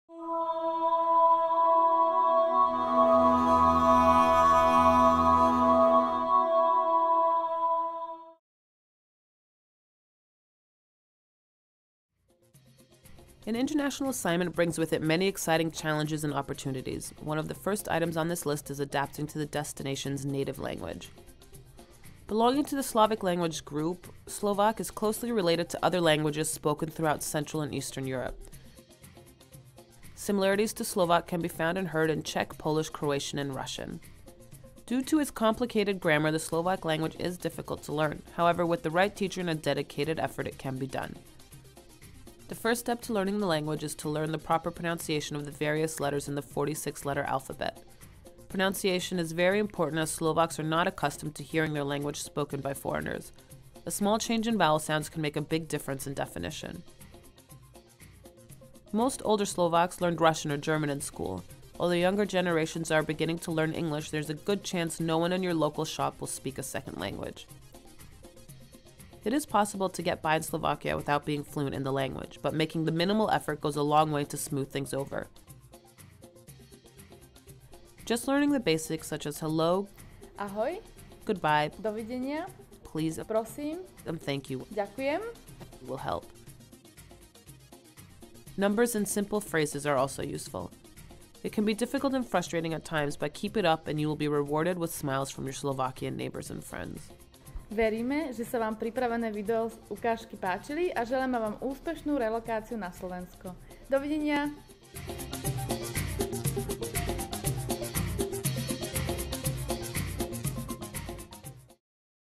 Living in Bratislava: The Slovak Language
Moving to a new country is difficult in itself. Learning to communicate in a new language can present another challenge, but once overcome can make your experience in your new country much more enjoyable. Slovak is the official language of the Slovak Republic. It is closely related to the Polish and Ukrainian languages and is mutually intelligible with the Czech language. With the difficult grammar and pronunciation, Slovak is a hard language to master, but learning simple phrases and numbers will improve your integration into local life.

If you are an expatriate, you will definitely enjoy our city guide videos